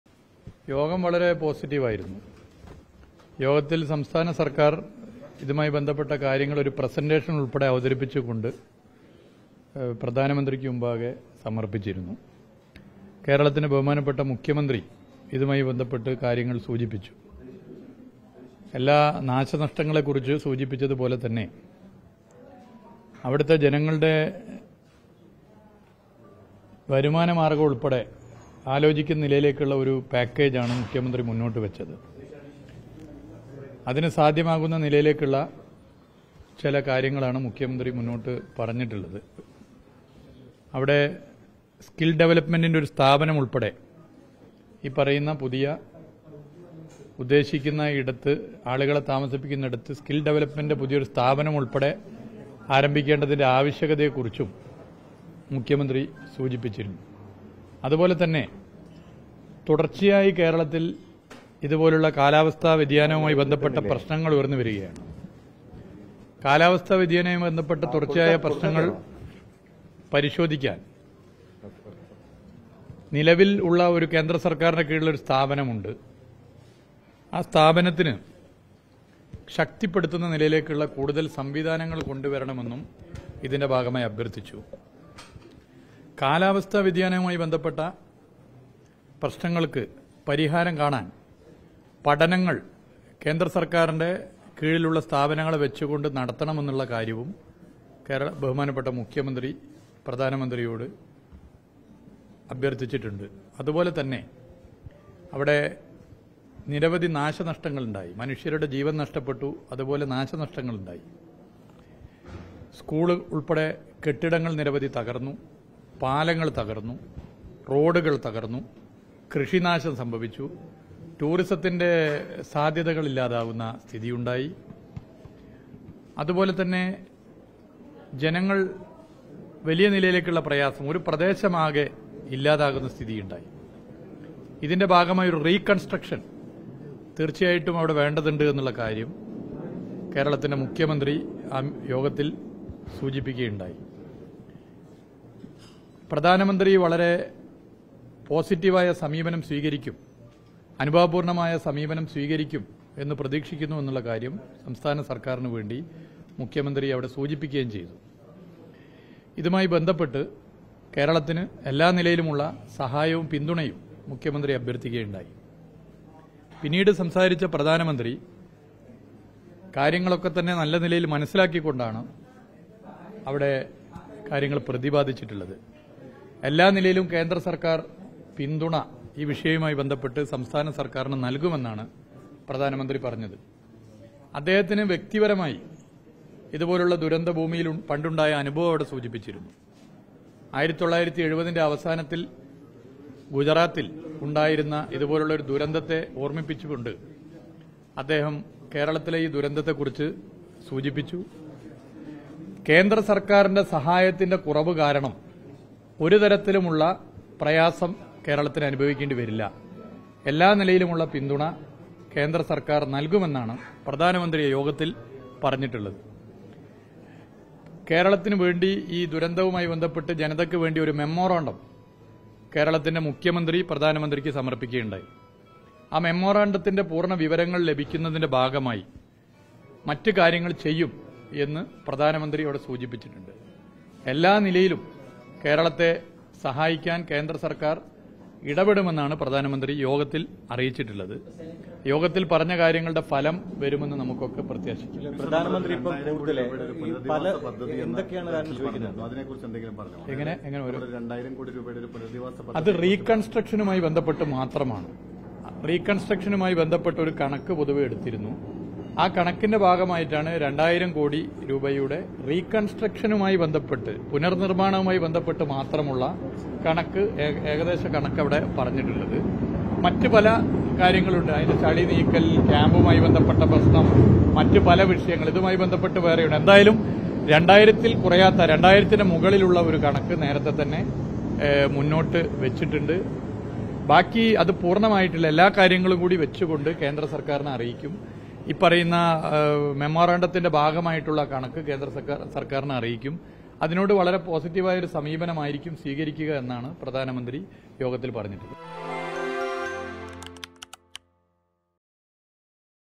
'നല്ല പിന്തുണ സംസ്ഥാന സര്‍ക്കാരിന് നല്‍കുമെന്നാണ് പ്രധാനമന്ത്രി പറഞ്ഞത്' | P A Mohammed Riyas
കേന്ദ്ര സഹായത്തിന്റെ കുറവ് കാരണം കേരളത്തിന് പ്രയാസം അനുഭവിക്കേണ്ടി വരില്ലെന്നാണ് പ്രധാനമന്ത്രി പറഞ്ഞത്'; പി എ മുഹമ്മദ് റിയാസ് | P A Mohammed Riyas

With Regards
Team RBC